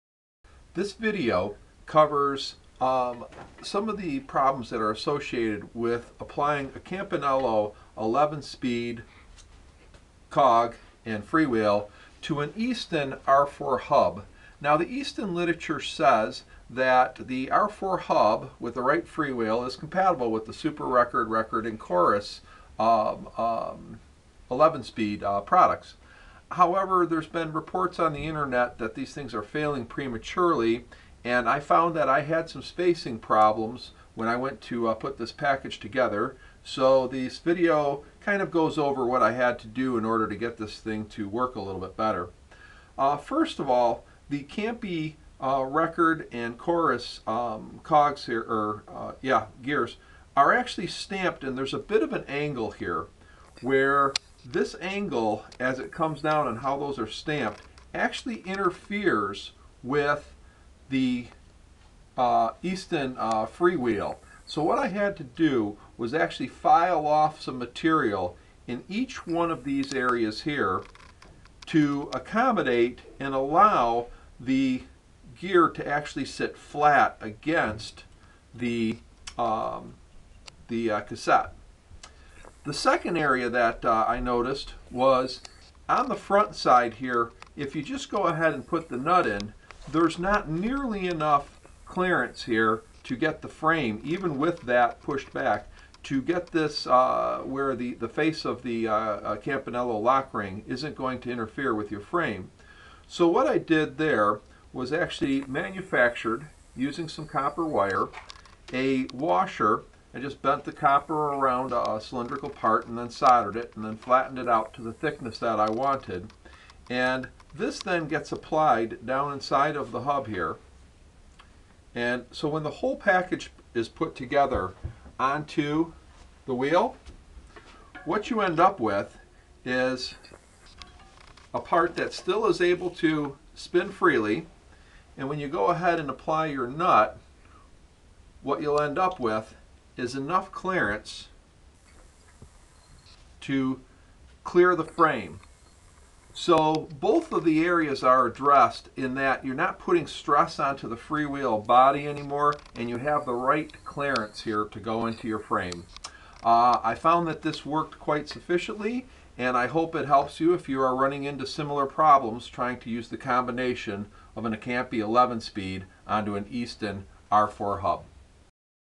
Easton R4 Hub with Campagnolo 11 Speed Driveline - Problem Solved!
This video identifies incompatibility issues between a machined Easton R4 freewheel (hub) and stamped Campagnolo 11 Speed Freewheel / Cog.  The issues may create premature failures and certainly cause frame clearence problems.  All problems are overcome with a file and custom washer.

Two clearance problems with Easton R4 hubs running Campy 11 Speed drivelines were identified and solved:

Problem #1: Taper in Campagnolo gear stamping interferes with the  Easton Freewheel machined lip.  This causes three syptoms, the freewheel is loaded and stressed which may cause the premature failutes, the 11th gear does not catch on the freebub by much and the Campy lockring hits the frame.

Solution = file the aluminum Easton Machined lip in the areas where the steel gear hits to allow the cog to fully seat agianst the freehub shoulder.

Problem #2:  Easton R4 hub nut is not long enough to exend past the Capmy lock ring.

Solution = add small copper washer between the nut and axel.  The overall spacing stays in tolerence for a standard spacing of 131mm on the frame.